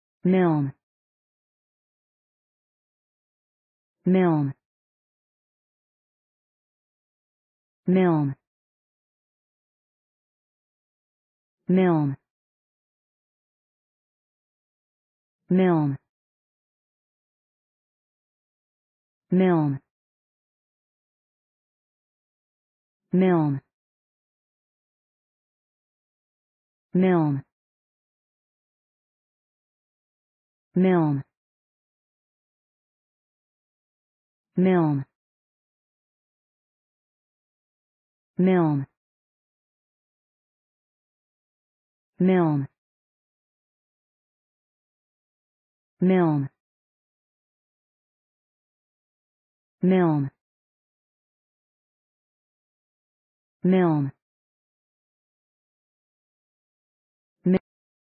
Pronunciation of Milne | Definition of Milne
Milne pronunciation | How to pronounce Milne in English?
/mɪln/
Meaning of Milne | What is Milne?
(noun) English writer of stories for children (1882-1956)
What is Milne | Milne meaning and definition?